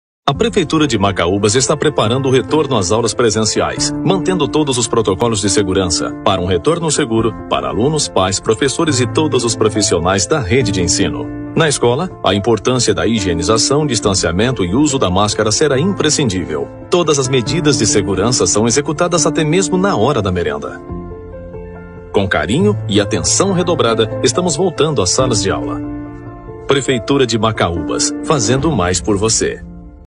SECRETARIA DE EDUCAÇÃO PREPARA A VOLTA ÀS AULAS PRESENCIAIS - 19/01/2022
A Secretaria de Educação, juntamente com a secretaria de saúde estão preparando tudo para uma volta as aulas mais segura para todos da rede municipal de ensino. A volta está prevista para o dia 14 de fevereiro.

Prefeitura de Macaúbas uma nova história, um Novo tempo.

Locução: Máximo
Imagens, Edição e Finalização: Plínio Figueiredo